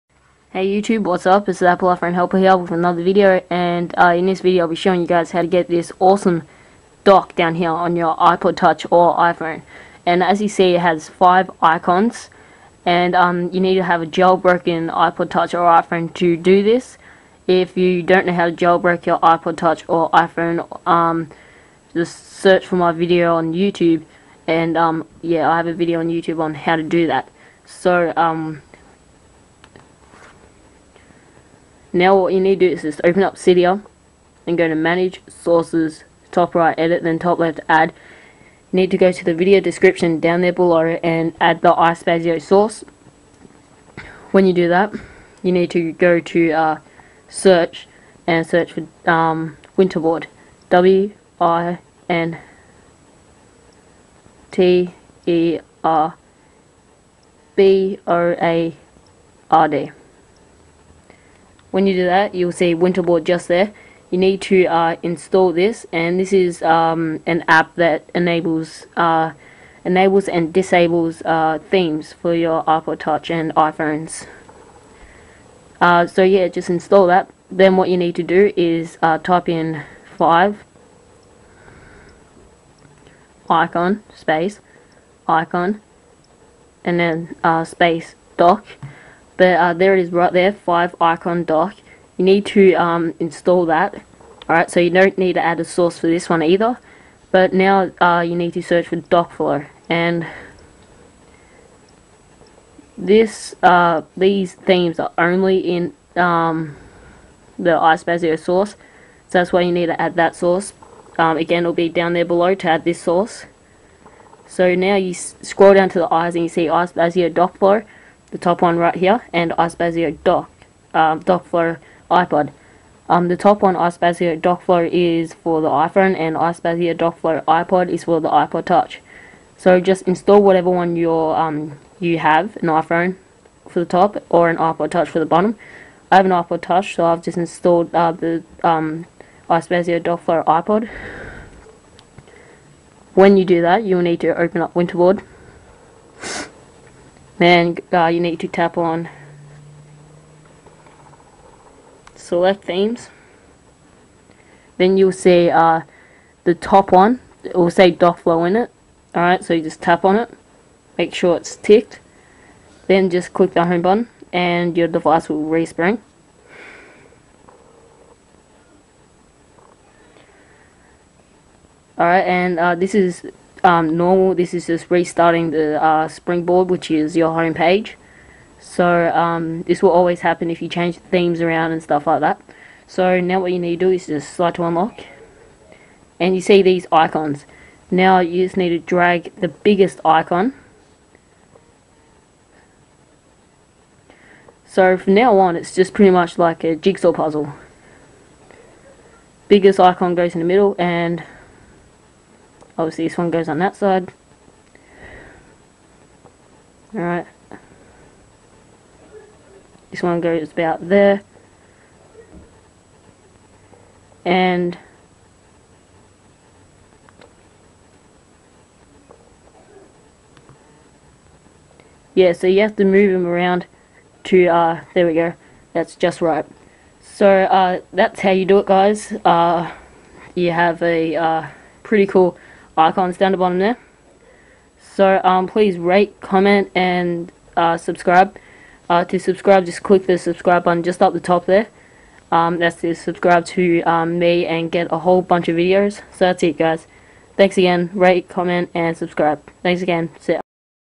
how to get a awesome dock for your iPod Touch and iPhone
hey youtube its theappleiphonehelper with another great video. In this video im going to show you how to get a awesome dock for your iPod Touch and iPhone.

1. Open cydia and then go to manage then sources then top right edit then top left add and add these 2 sources below.

2. Go to search and search for "Winterboard" and install. Go to search again and search for "five icon dock" tap on it and install.

3. Last time go to search and searck for "dockflow"

4. Scroll down to the "i" and you will see iSpazio DockFlow and you will see iSpazio Dockflow iPod.

5. If you have an iPod Touch install iSpazio DockFlow iPod.

6. If you have an iPhone install iSpazio Dockflow.

7. Once you have done that, Open winterboard and tap on the top one and click the home button.

8. Wait for your device to respring. When your device has done that slide to unlock then just put the icon in the position they belong.

Thanks alot for watching and i hoped i helped thanks everyone cya.